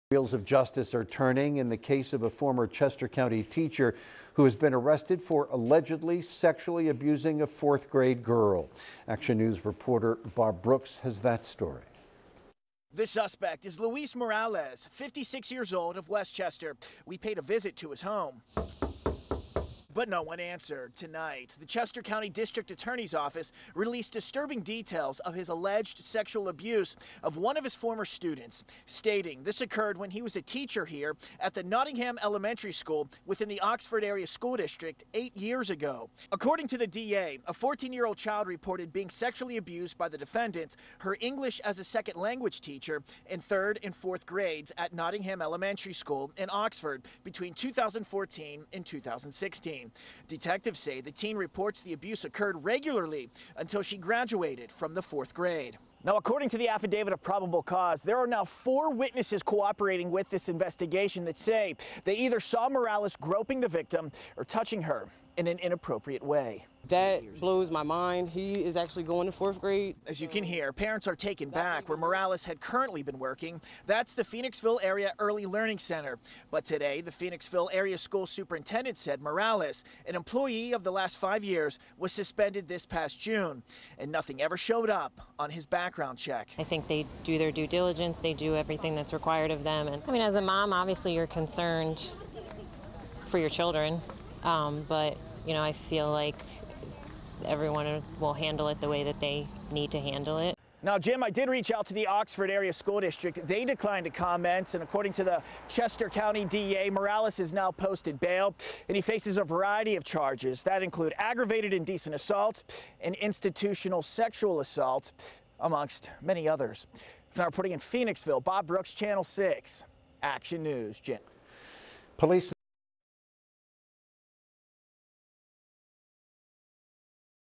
Former teacher accused of sexually abusing student in 3rd, 4th grade
A Chester County, Pennsylvania teacher is facing charges for the alleged sexual abuse of a student.

Authorities have arrested Luis Morales, 56, of West Chester, for allegedly sexually abusing a Nottingham Elementary School student while she was in the third and fourth grade. The alleged incidents happened between 2014 and 2016.

In June 2021, the 14-year-old victim reported the abuse to authorities. The child told Oxford Borough Police that Morales, who was her ESL teacher at the time, asked her to stay after school and that he touched her chest and genitals on multiple occasions.